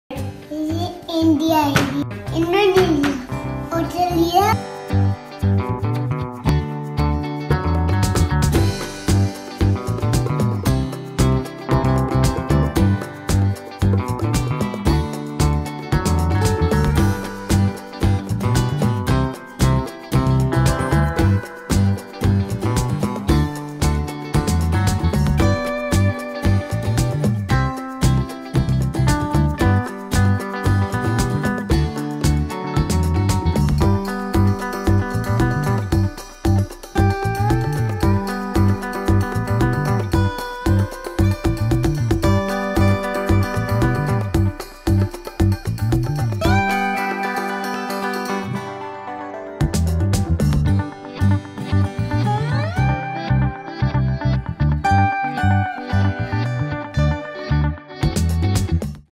Mapology Ad 59-sec
Learning Geography at home is even easier and more fun with our Mapology range of products that serves a perfect holistic educational aid for your child's development! Here's a glimpse of the appreciation, love & positive feedback that this range has received globally.